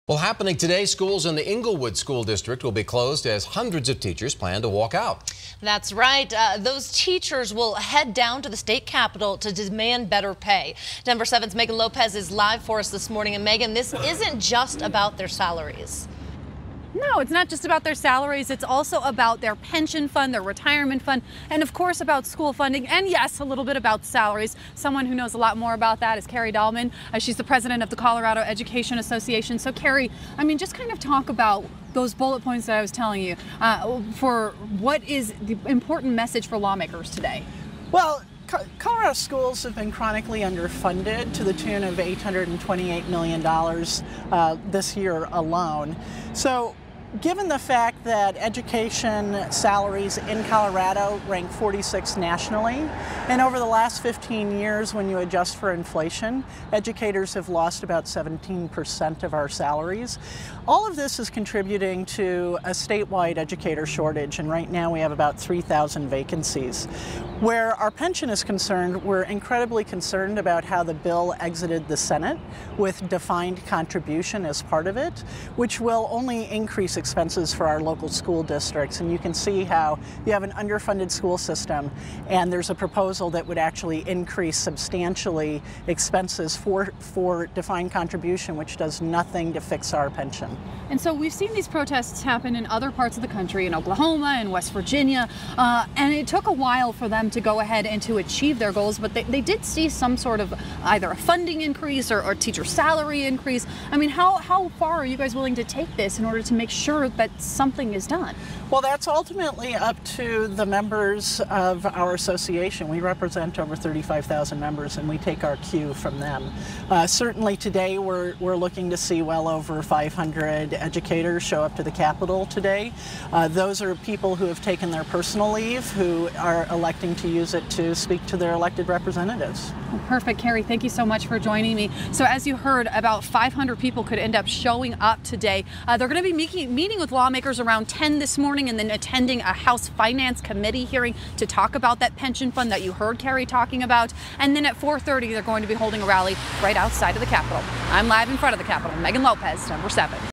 Colorado teachers plan Monday walkout to call for higher pay, better funding
Teachers across Colorado plan to walk out of school Monday to call attention to teacher pay and other issues.